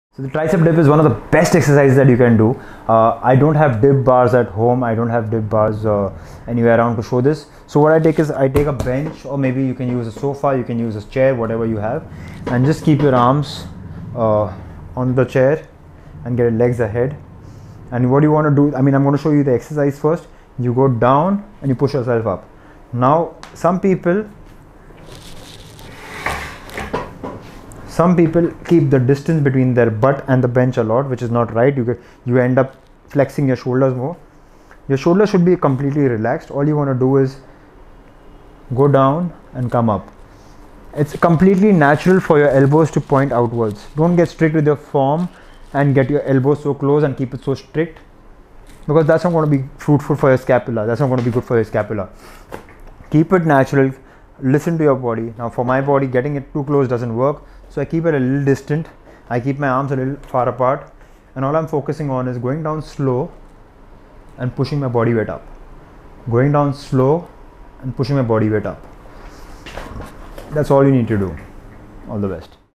TRICEP DIPS
Instructions:
1) When performing dip on the bench, keep your elbows pointing straight behind you.

2) Keep your body as close to the bench as possible as you dip down.